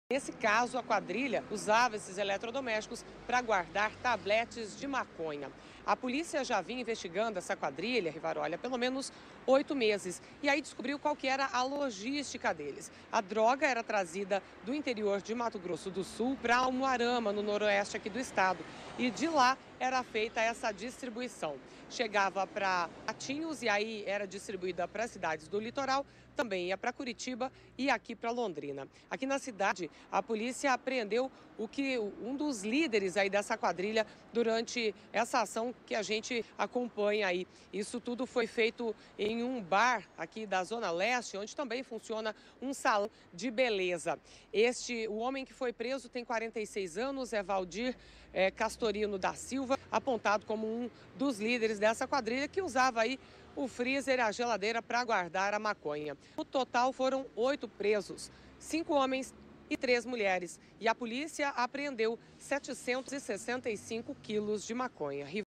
Quadrilha usava congelador para guardar tabletes de maconha
A droga era trazida do interior do Mato Grosso do Sul para Umuarama, no noroeste do Estado, onde toda a distribuição era realizada. O casal que comandava o esquema foi preso em Matinhos e está à disposição da justiça.